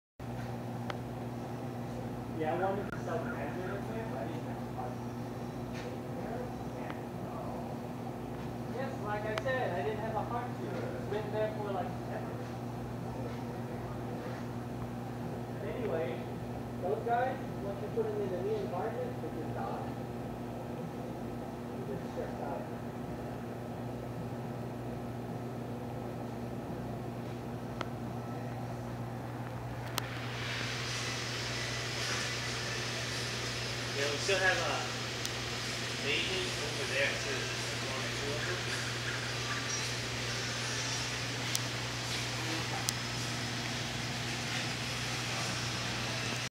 Sand Sifting Goby and Duncan Coral
A fun Diamond Watchman Goby at the local fish store.